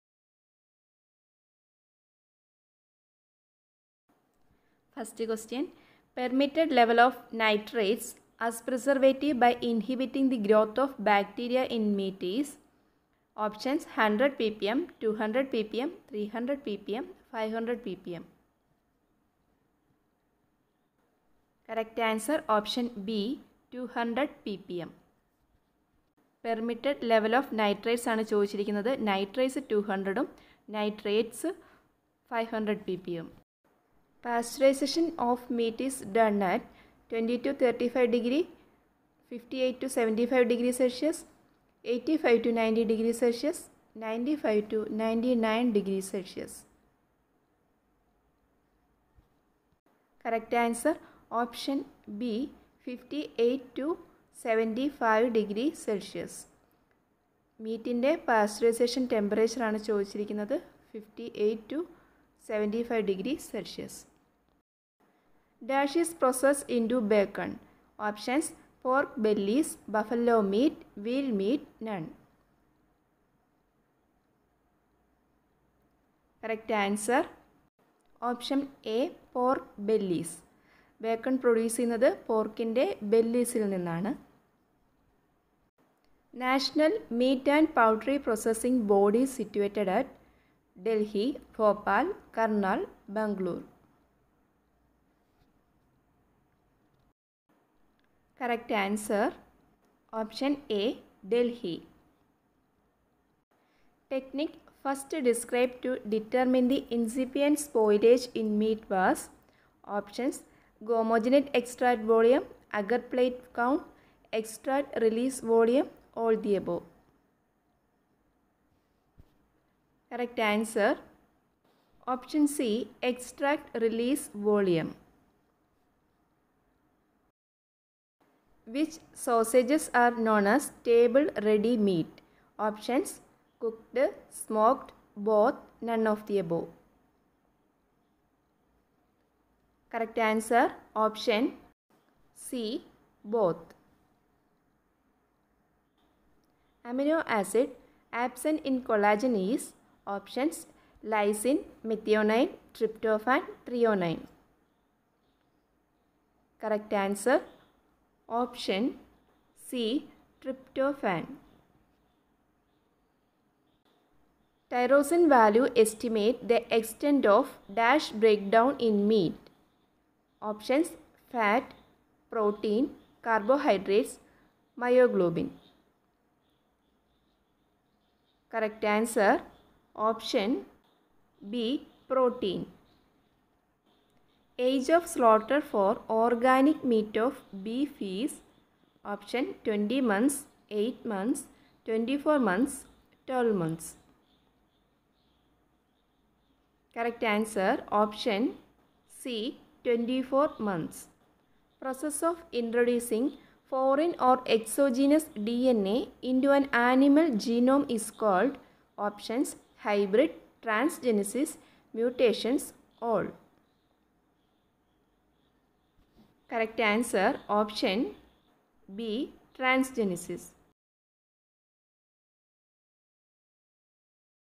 VETERINARY SCIENCE MCQ fssai fso kpsc uppsc
veterinary science mcqs for various exams like kerala food safety officer, Uttarpradesh psc,fssai examinations